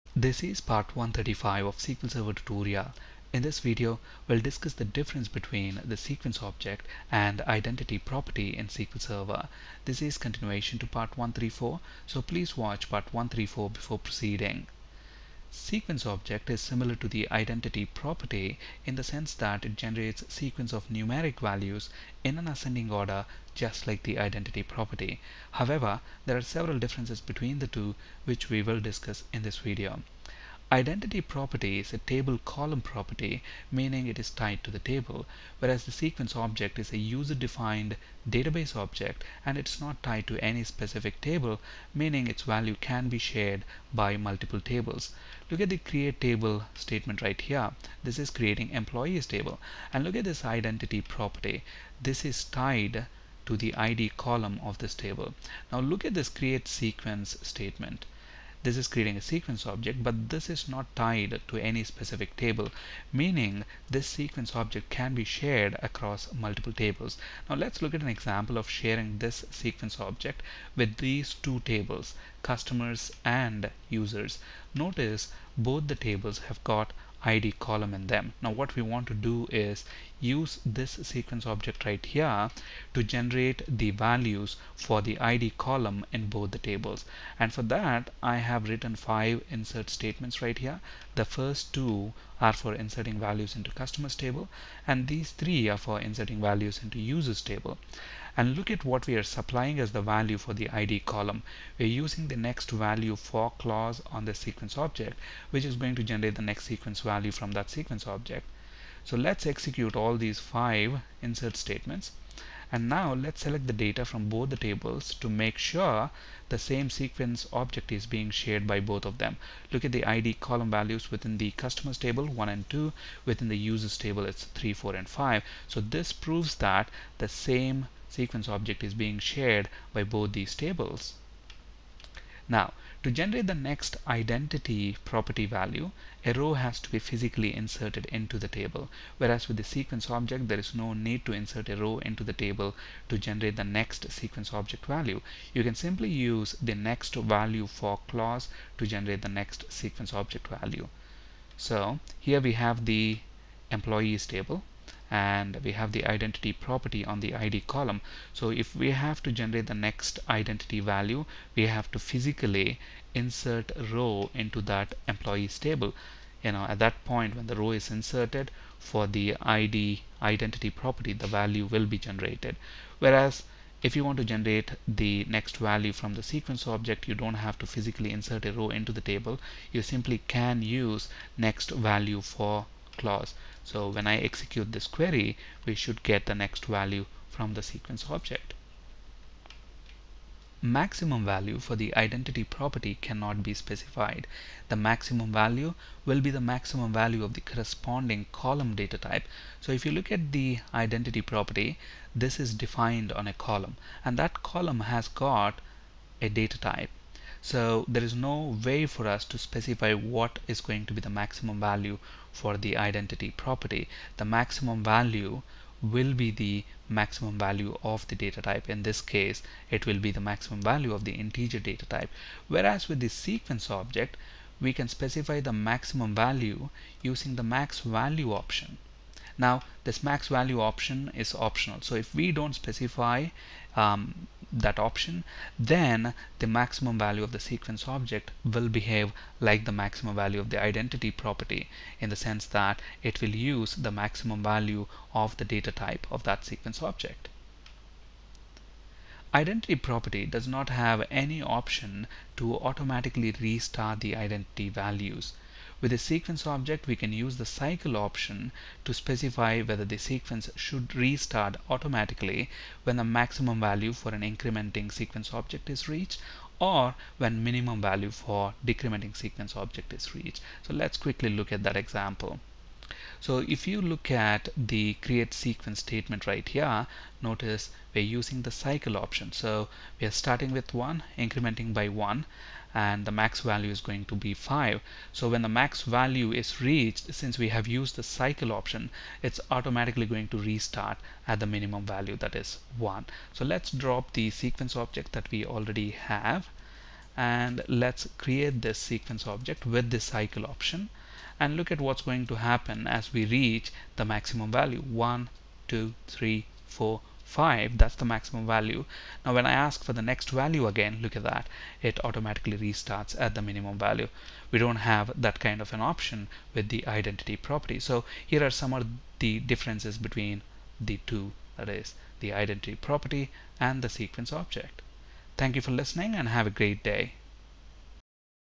Difference between sequence and identity in SQL Server
sql server identity vs sequence

In this video we will discuss the difference between SEQUENCE and IDENTITY in SQL Server

Sequence object is similar to the Identity property, in the sense that it generates sequence of numeric values in an ascending order just like the identity property. However there are several differences between the 2 which we will discuss in this video.

Identity property is a table column property meaning it is tied to the table, where as the sequence is a user-defined database object and is not tied to any specific table meaning it's value can be shared by multiple tables.

Example : Identity property tied to the Id column of the Employees table.

CREATE TABLE Employees
 Id INT PRIMARY KEY IDENTITY(1,1),
 Name NVARCHAR(50),
 Gender NVARCHAR(10)

Example : Sequence object not tied to any specific table

CREATE SEQUENCE [dbo].[SequenceObject]
AS INT
START WITH 1
INCREMENT BY 1

This means the above sequence object can be used with any table.

Example : Sharing sequence object value with multiple tables.

Step 1 : Create Customers and Users tables

CREATE TABLE Customers
 Id INT PRIMARY KEY,
 Name NVARCHAR(50),
 Gender NVARCHAR(10)
GO

CREATE TABLE Users
 Name NVARCHAR(50),
 Gender NVARCHAR(10)
GO

Step 2 : Insert 2 rows into Customers table and 3 rows into Users table. Notice the same sequence object is generating the ID values for both the tables.

INSERT INTO Customers VALUES (NEXT VALUE for [dbo].[SequenceObject], 'Ben', 'Male')
INSERT INTO Customers VALUES (NEXT VALUE for [dbo].[SequenceObject], 'Sara', 'Female')

INSERT INTO Users VALUES (NEXT VALUE for [dbo].[SequenceObject], 'Tom', 'Male')
INSERT INTO Users VALUES (NEXT VALUE for [dbo].[SequenceObject], 'Pam', 'Female')
INSERT INTO Users VALUES (NEXT VALUE for [dbo].[SequenceObject], 'David', 'Male')
GO

Step 3 : Query the tables
SELECT * FROM Customers
SELECT * FROM Users
GO

Output : Notice the same sequence object has generated the values for ID columns in both the tables

To generate the next identity value, a row has to be inserted into the table, where as with sequence object there is no need to insert a row into the table to generate the next sequence value. You can use NEXT VALUE FOR clause to generate the next sequence value.

Example : Generating Identity value by inserting a row into the table

INSERT INTO Employees VALUES ('Todd', 'Male')

Example : Generating the next sequence value using NEXT VALUE FOR clause.

SELECT NEXT VALUE FOR [dbo].[SequenceObject]

Maximum value for the identity property cannot be specified. The maximum value will be the maximum value of the correspoding column data type. With the sequence object you can use the MAXVALUE option to specify the maximum value. If the MAXVALUE option is not specified for the sequence object, then the maximum value will be the maximum value of it's data type.

Example : Specifying maximum value for the sequence object using the MAXVALUE option

CREATE SEQUENCE [dbo].[SequenceObject]
START WITH 1
INCREMENT BY 1
MAXVALUE 5

CYCLE option of the Sequence object can be used to specify whether the sequence should restart automatically when the max value (for incrementing sequence object) or min value (for decrementing sequence object) is reached, where as with the Identity property we don't have any such option to automatically restart the identity values.

Example : Specifying the CYCLE option of the Sequence object, so the sequence will restart automatically when the max value is exceeded

CREATE SEQUENCE [dbo].[SequenceObject]
START WITH 1
INCREMENT BY 1
MINVALUE 1
MAXVALUE 5
CYCLE